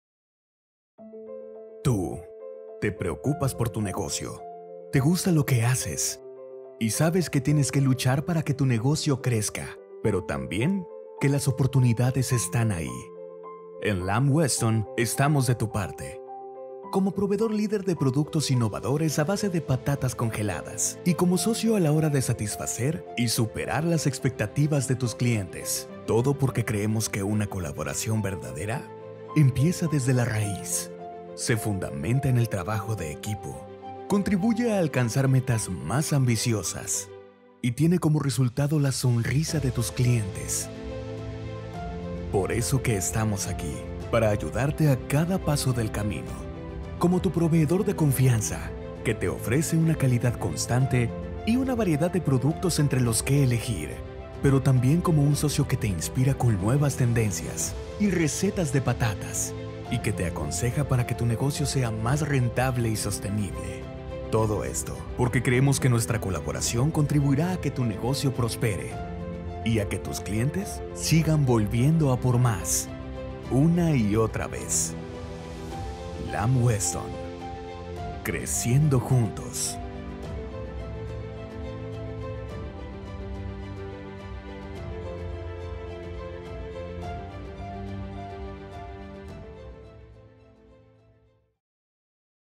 Patatas que cuidan tu negocio
Como proveedor, Lamb Weston es líder en patatas fritas congeladas y otros productos de patata. En nuestra labor como socio iremos más allá para apoyar tu negocio de restauración.